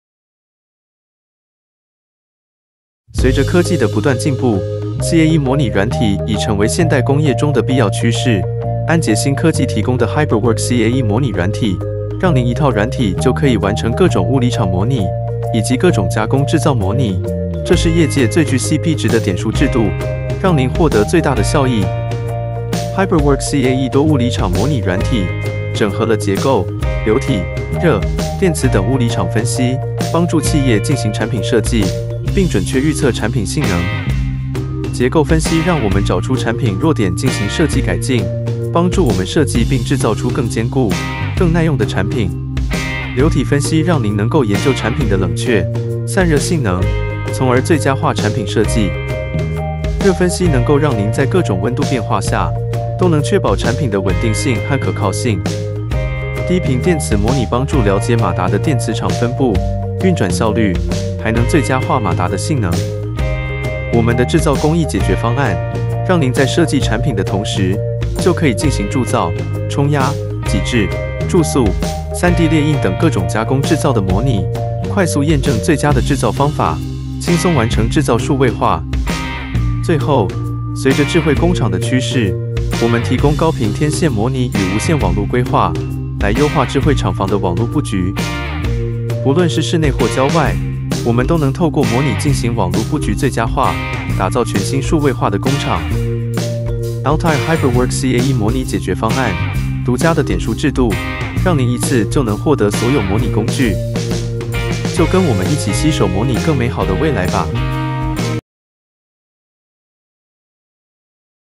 Altair HyperWorks CAE 多物理場模擬軟體系列介紹 | 最具 CP 值的點數制度 | 結構分析, 流體動力學, 熱分析, 低頻馬達, 高頻天線, 製造工藝模擬 - 安捷新科技
HyperWorks 解決方案平台，可為所有工程師提供解決方案，包括基於模型的系統設計以及從早期幾何構思到詳細多物理場模擬優化。

AgileSim Technology Corp.